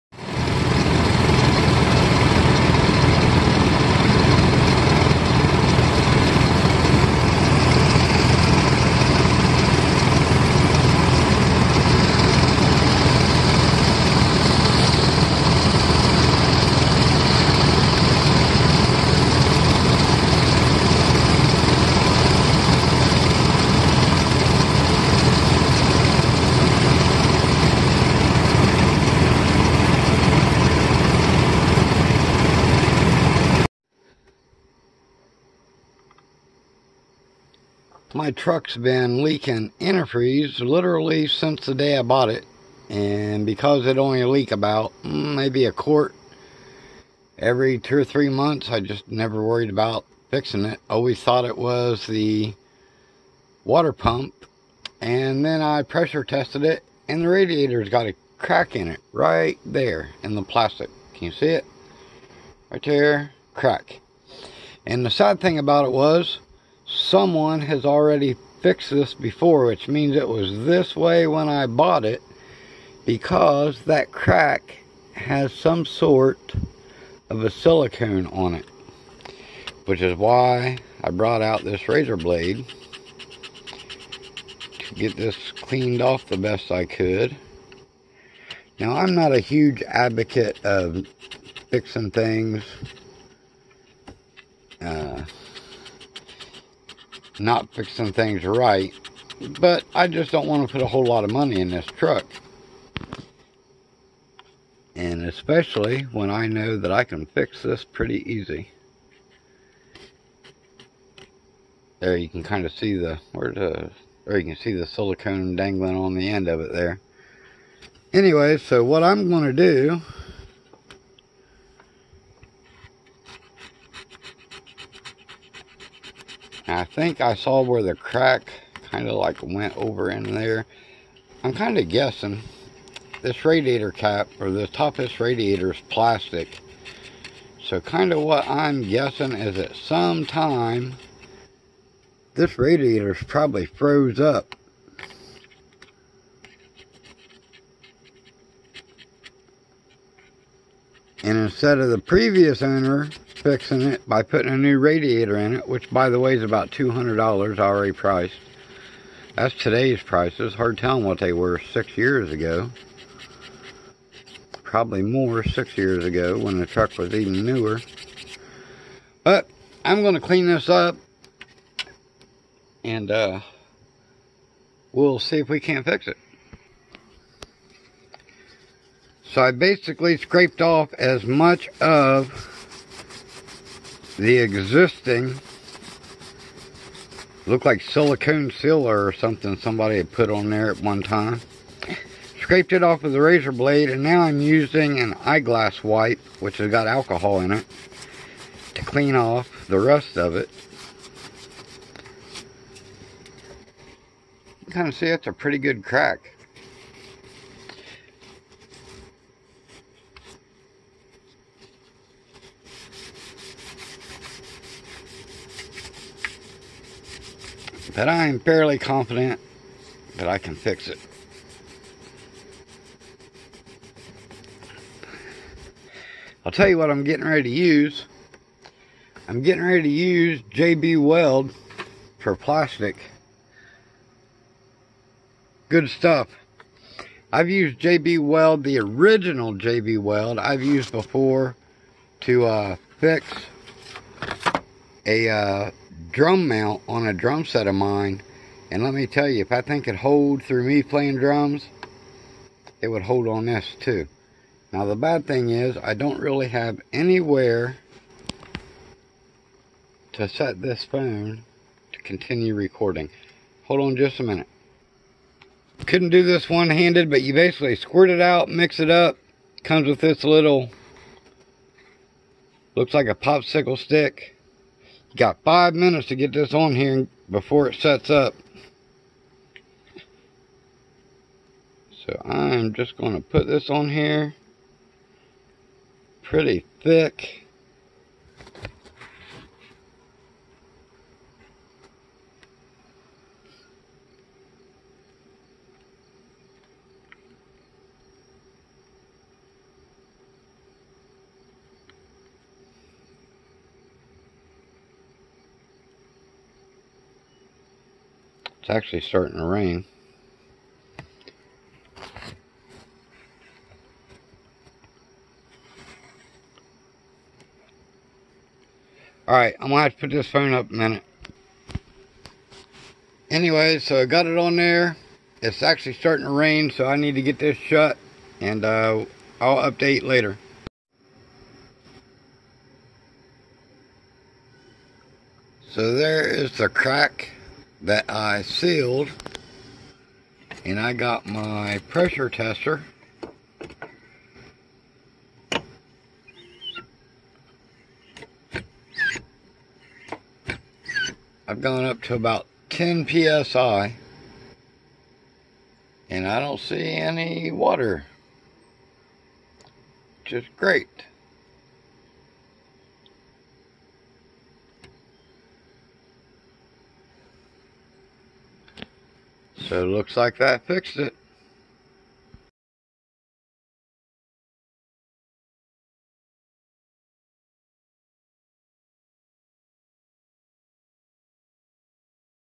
fixing homestead truck: a frugal $8 vs $200+
I needed to fix my truck cooling system. Its been leaking for 6 years. I originally thought it was the water pump but turns out its the radiator. I could order the parts and fix it myself for about $200 and a couple hours of time, or i could pay retail price plus labor to let a garage fix it (estimated about $700), or i can spend $8 and 5 minutes to fix it. guess which choice frugal me chose?